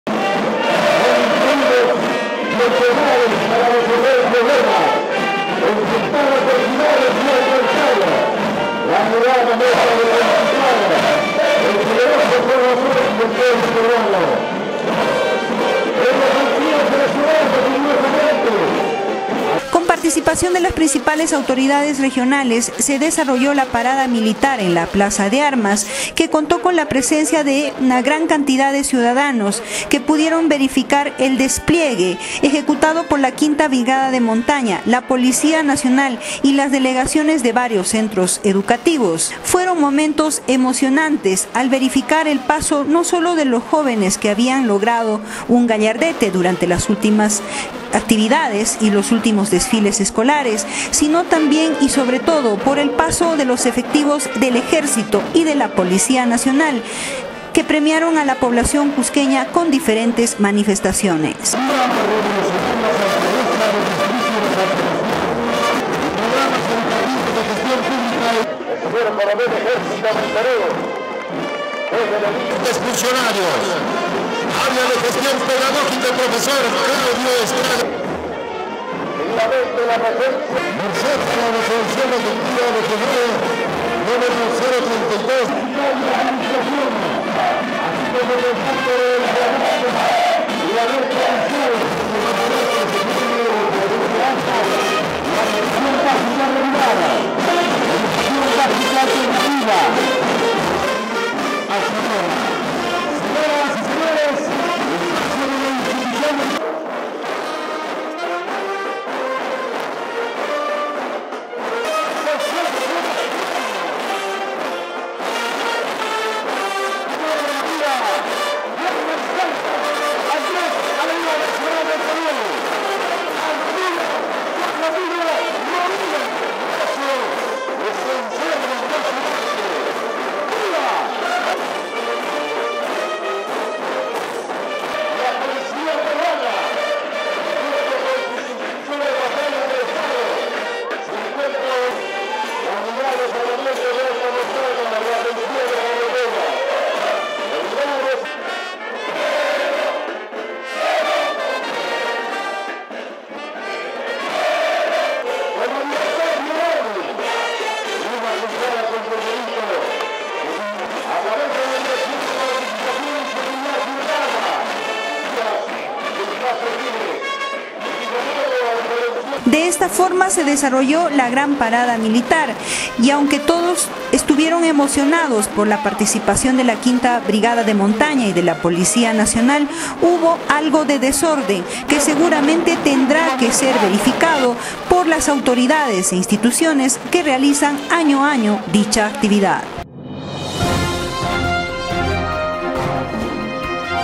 26 julio 2016 DESFILE Y PARADA MILITAR, SE REALIZO EN LA PLAZA DE ARMAS DEL CUSCO
CON LA PARTICIPACIÓN DE LAS PRINCIPALES AUTORIDADES REGIONALES SE DESARROLLÓ LA GRAN PARADA Y DESFILE MILITAR EN LA PLAZA DE ARMAS … MIEMBROS DE LA QUINTA BRIGADA DE MONTAÑA Y LA POLICIA NACIONAL ARRANCARON MAS DE UN APLAUSO A LOS ASISTENTES PESE A UNA EVIDENTE DESORGANIZACION Y FALTA DE PROTOCOLO …